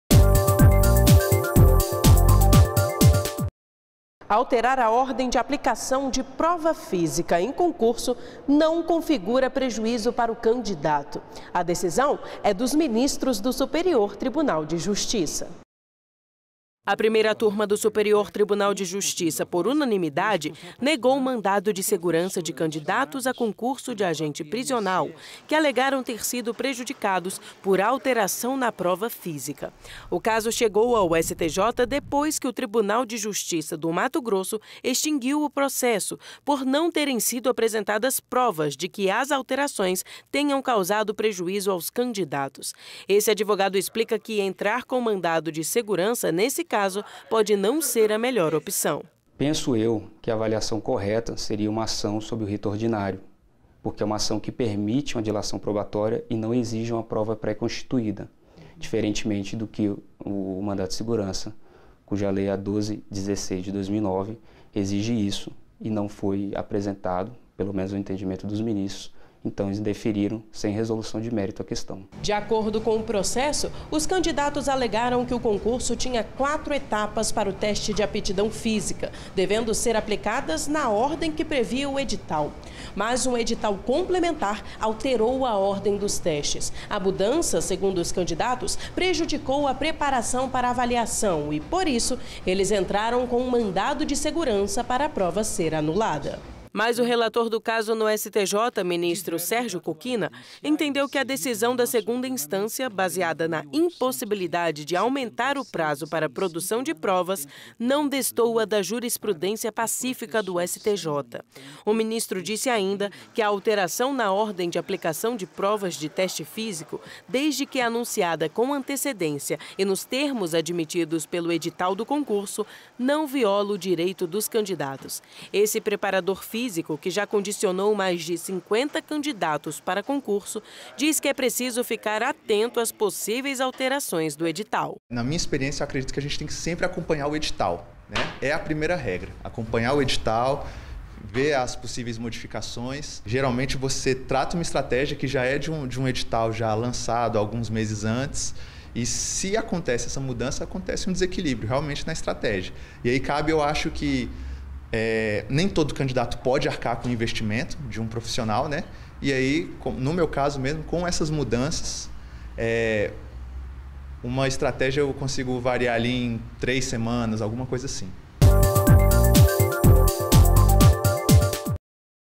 Clique STJ - Prova Física de Concurso
Alterar a ordem de aplicação de prova física de concurso não configura prejuízo para o candidato. A decisão é dos ministros do Superior Tribunal de Justiça.